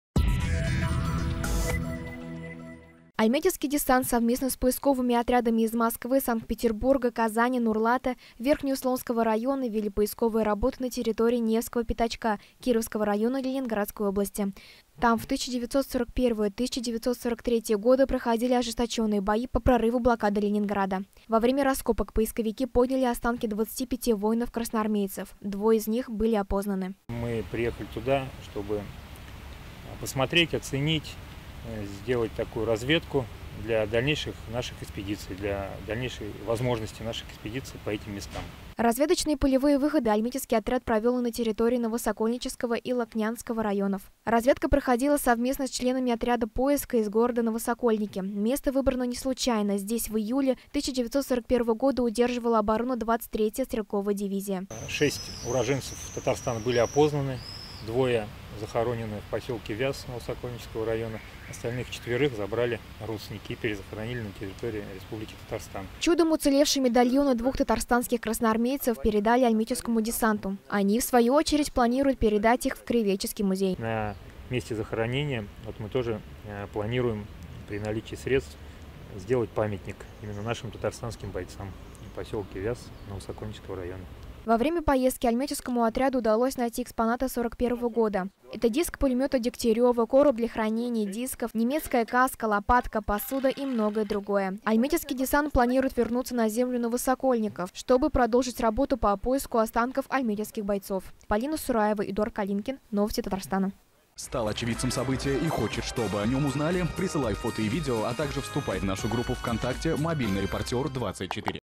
Альметьевские поисковики вернулись из Ленинградской области с ценными находками
Поисковый отряд «Десант» в составе шести человек вернулся из разведочной экспедиции.